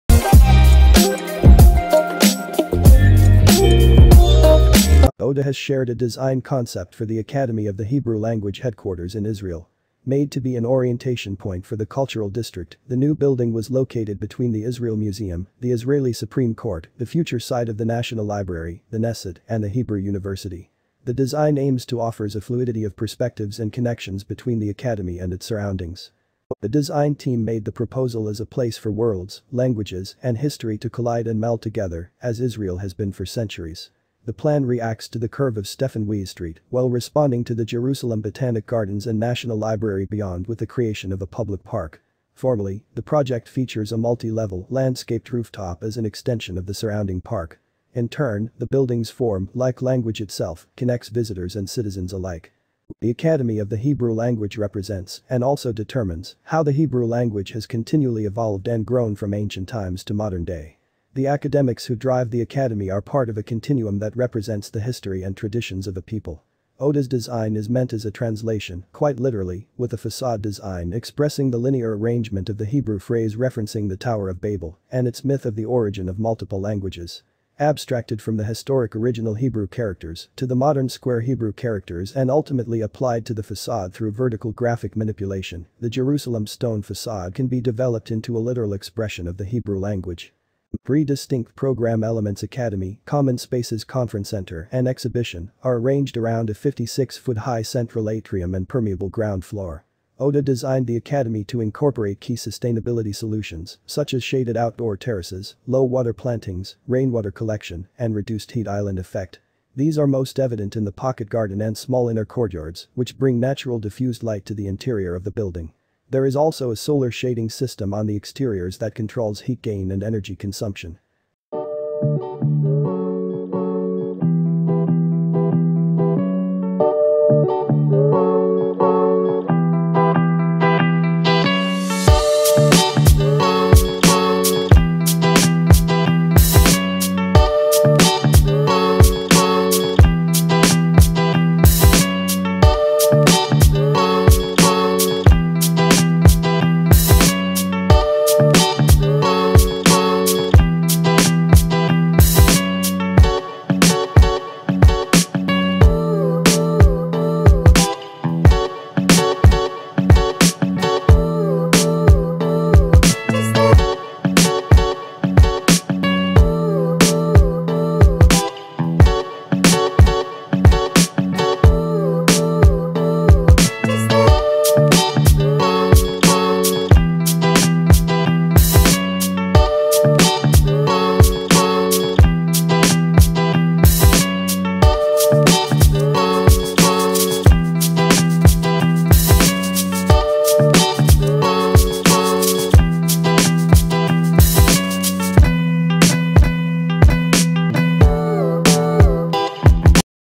Design Concept for Academy of the Hebrew Language Headquarter in Israel | New Buildings of 2021
ODA has shared a design concept for the Academy of the Hebrew Language headquarters in Israel. Made to be an orientation point for the cultural district, the new building was located between the Israel Museum, the Israeli Supreme Court, the future site of the National Library, the Knesset, and the Hebrew University.

Design Concept for Academy of the Hebrew Language